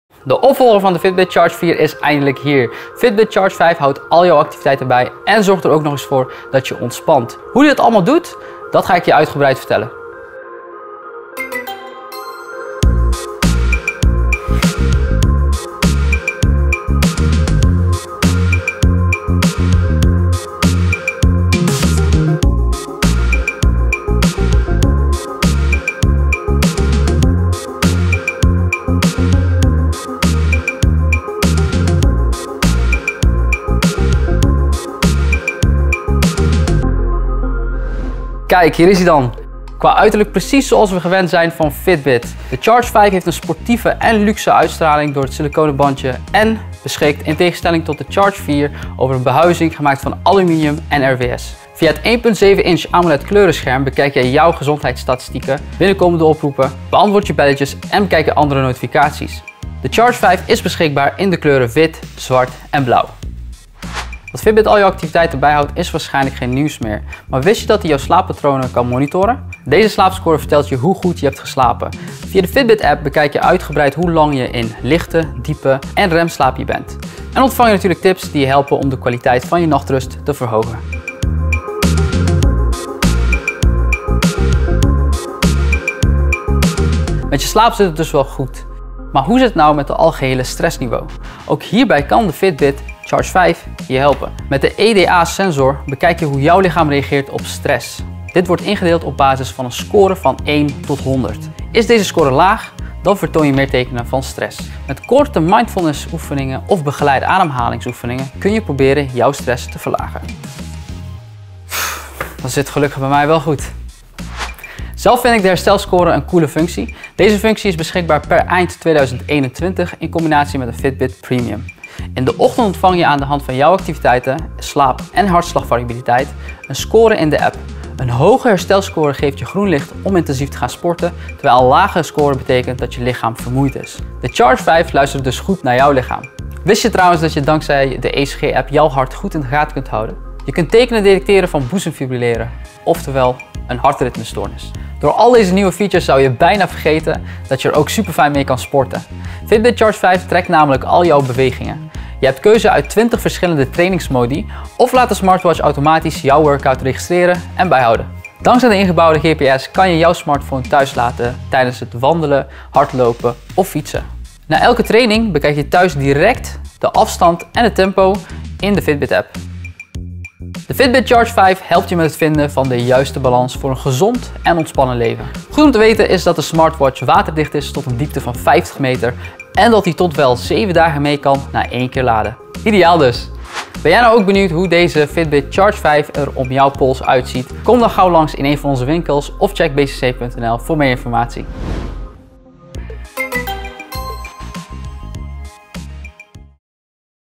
Unboxing: Fitbit Charge 5
De opvolger van Fitbit Charge 4 is eindelijk hier. Fitbit Charge 5 houdt al jouw activiteiten bij en zorgt er ook nog eens voor dat je ontspant. Hoe ‘ie dat allemaal doet? Dat ga ik je uitgebreid vertellen!

Benieuwd naar ons aanbod? Bekijk hier Fitbit Charge 5